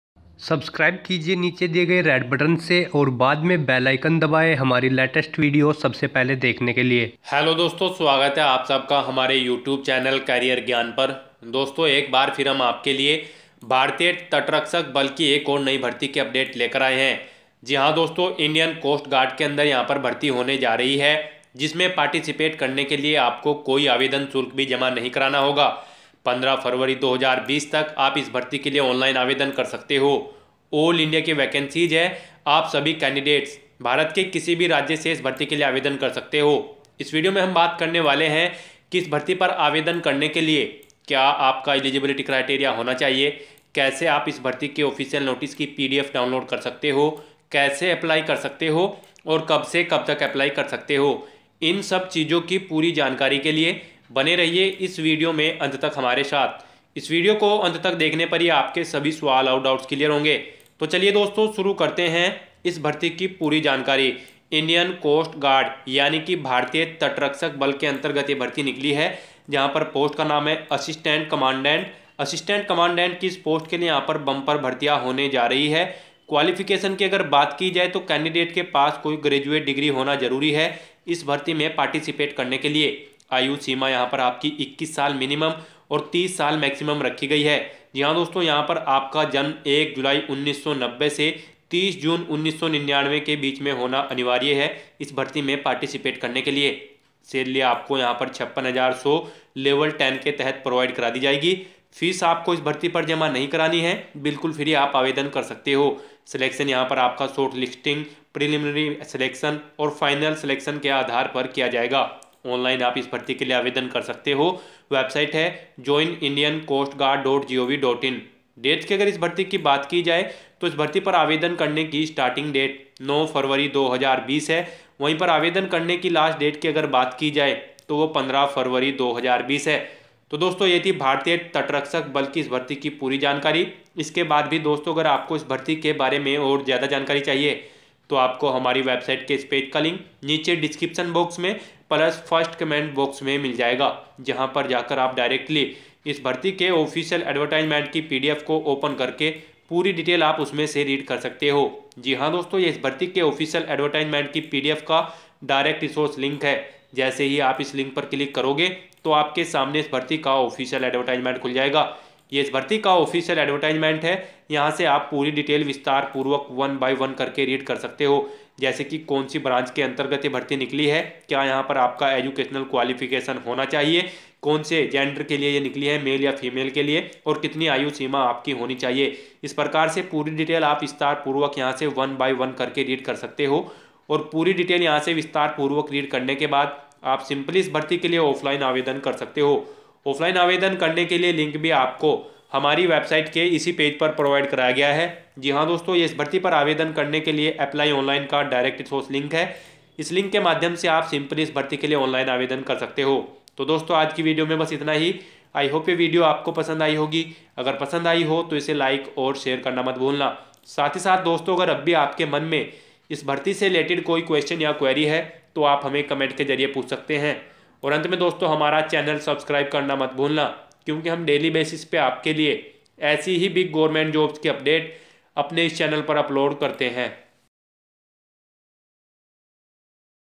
भारतीय तटरक्षक बल में निकली बड़ी भर्ती (Indian Coast Guard Recruitment 2020)
Hey guys, here in this video we are providing you details about Indian Coast Guard Recruitment 2020 latest update. Recently application forms are inviting from Indian national eligible candidates by Indian Coast Guard for fill up the post of Assistant Commandant. Through this recruitment notification there are Large Numbers of vacancies will be fulfilled. Candidates who are eligible and liable for this recruitment opportunity, they can apply on or before last date. For more information related to this recruitment opportunity like eligibility criteria, selection process, application fee details, so please see this video till the end.